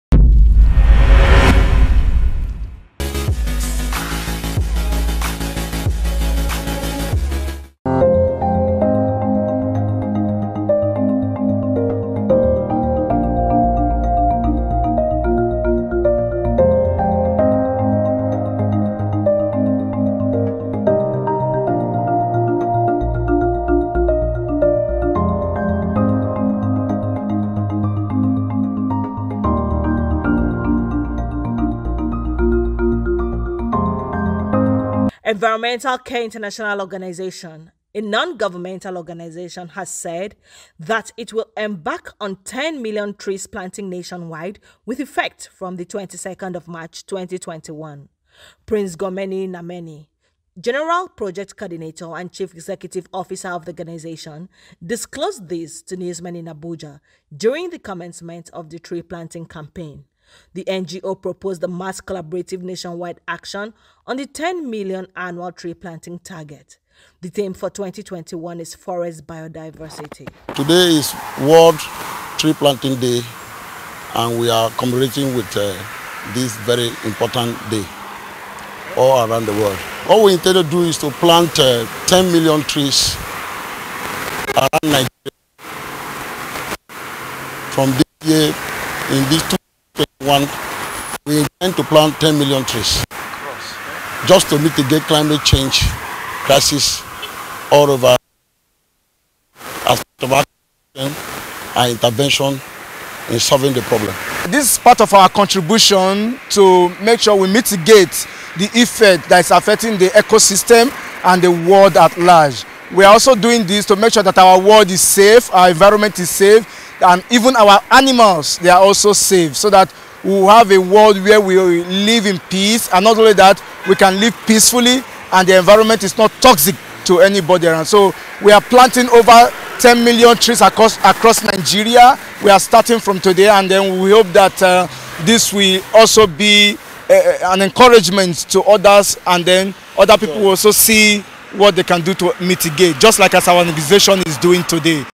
NGO Embarks On Nationwide Tree Planting In Nigeria …Set to Plant 10 million Trees Nationwide
A Non-Governmental Organization, Environmental Care International Organization (ECIO)
 has said,  that it will embark on ten million trees planting nationwide with effect from  22nd of March 2021.